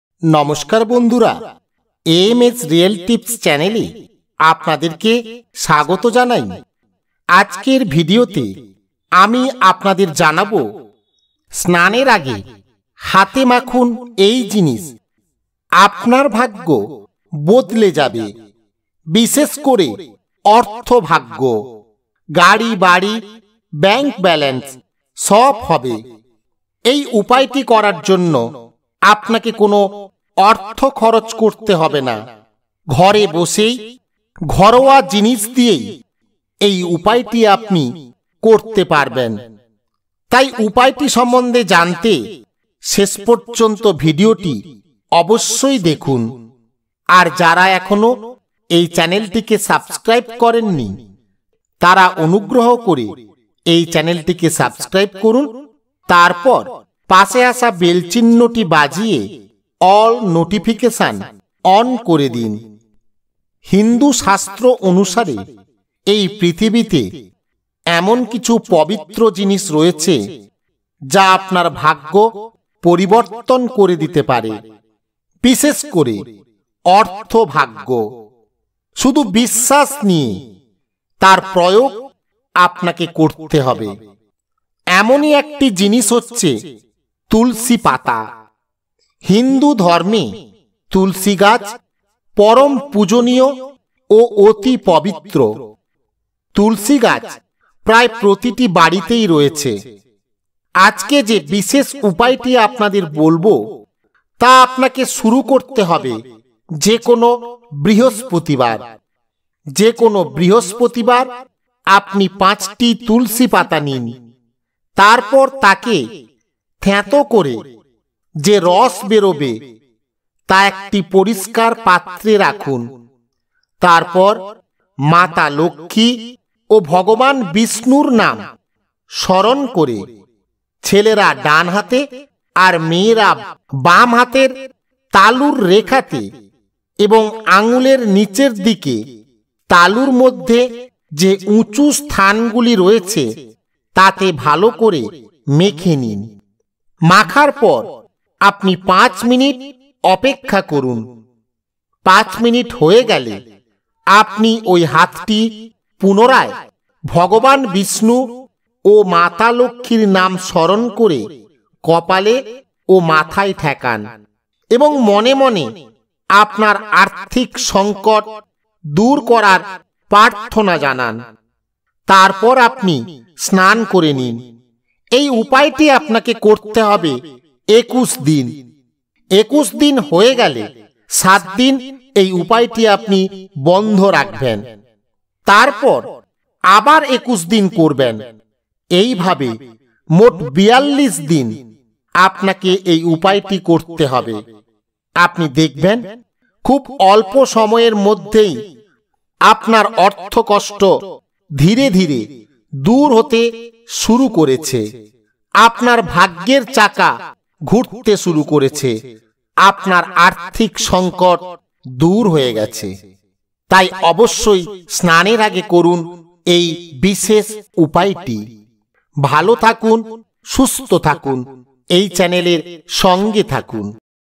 স্নান করার আগে হাতে মাখুন এই জিনিসটি। আপনার ভাগ্য বদলে যাবে।
আজকের ভিডিওতে আমি আপনাদের জানাবো ভাগ্য পরিবর্তনের এক বিশেষ উপায়। এই উপায়টি করতে আপনার কোনো টাকা পয়সা খরচ হবে না। শুধু মনে ভক্তি ও বিশ্বাস নিয়ে কাজটি আপনাকে করতে হবে। তাই স্নান করার আগে হাতে মাখুন এই জিনিস। আপনার ভাগ্য বদলে যাবে। গাড়ী বাড়ী ব্যাঙ্ক ব্যালেন্স সব হবে।